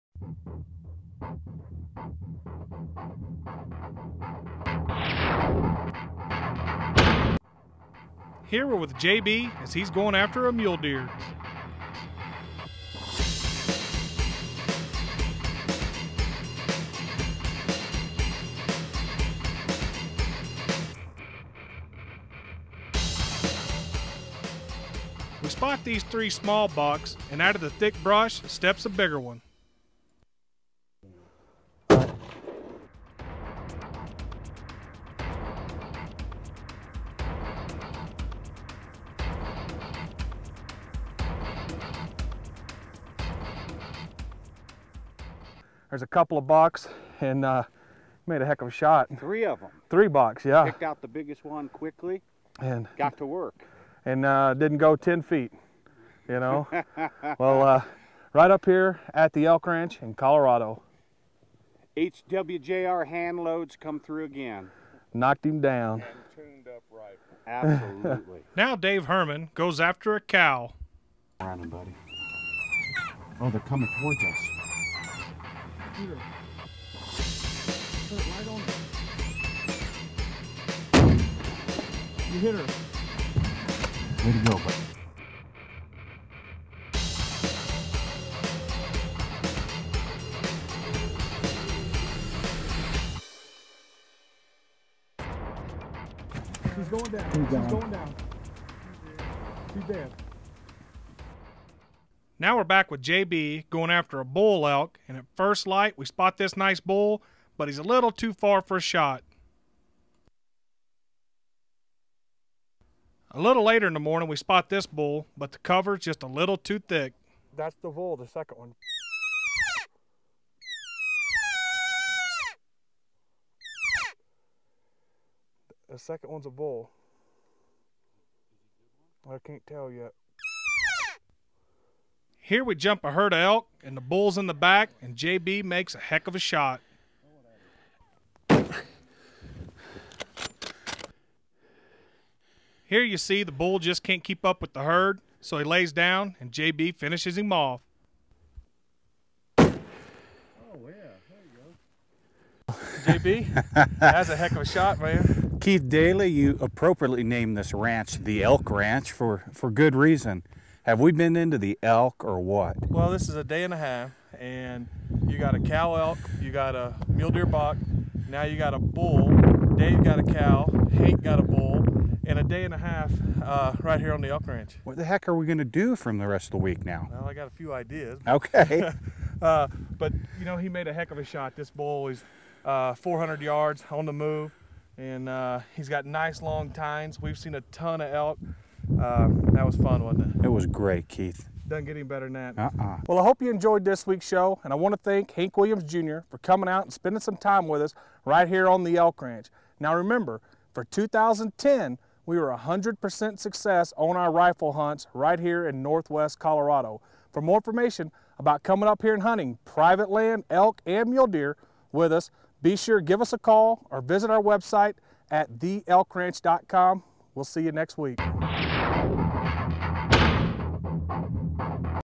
JB & Dave at The Elk Ranch
JB and Dave hunt for mule deer and elk at The Elk Ranch with Keith Dailey from Arctic Cat Outdoors TV.